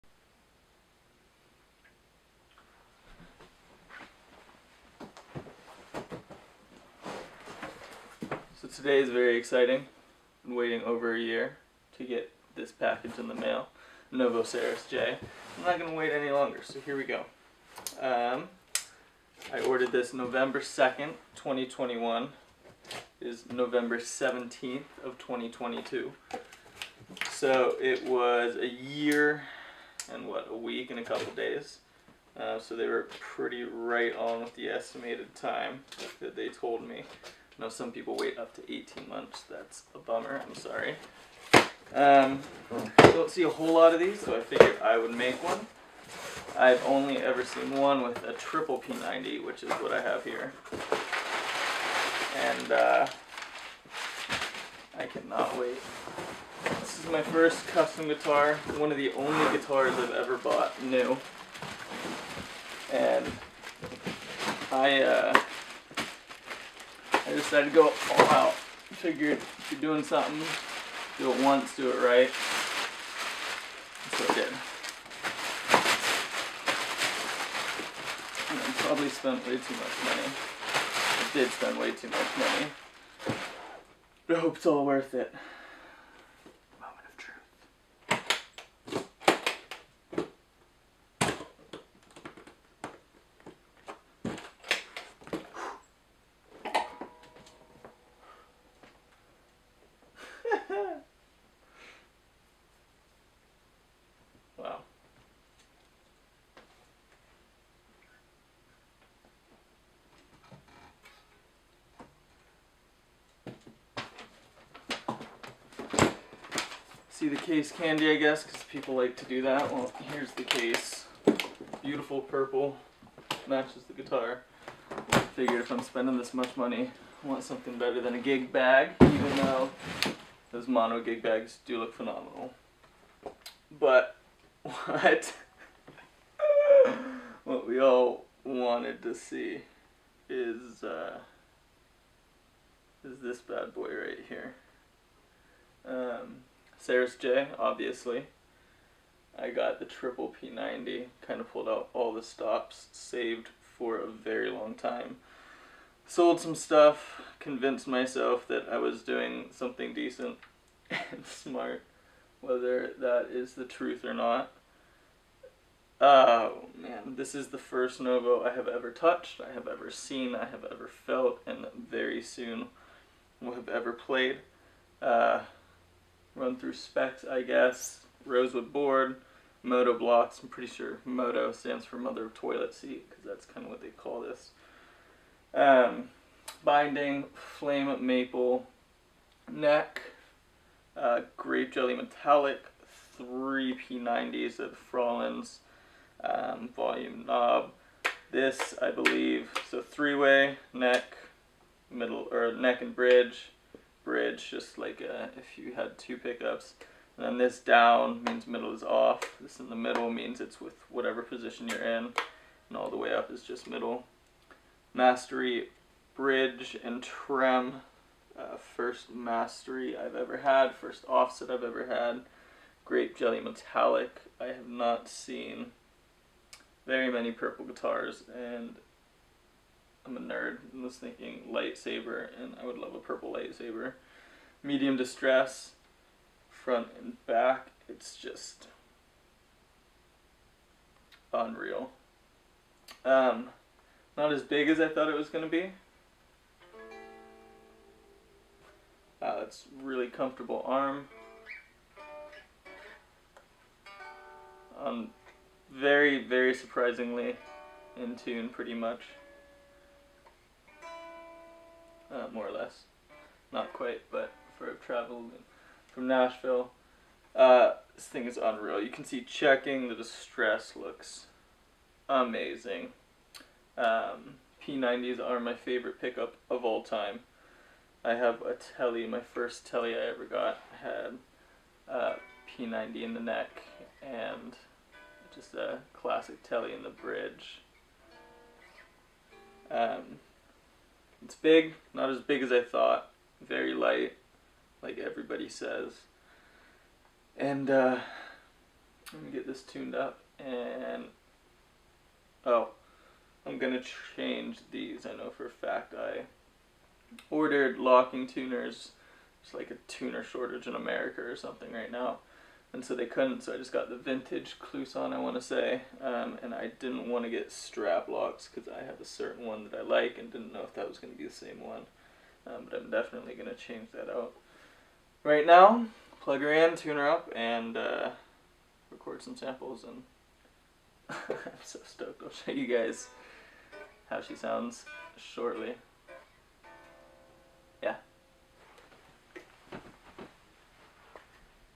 NOVO Serus J Unboxing and First Impressions
Unboxing and spec check of a brand new custom Novo Serus J with triple p90s, MOTO blocks, and binding with first impressions.  Demo video coming soon!